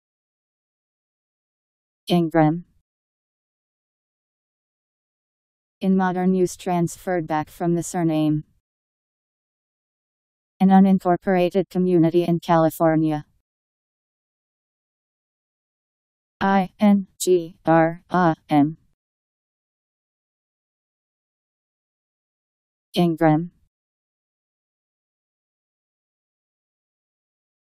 Ingram Meaning
Video shows what Ingram means. , in modern use transferred back from the surname.. An unincorporated community in California.  Ingram Meaning. How to pronounce, definition audio dictionary. How to say Ingram. Powered by MaryTTS, Wiktionary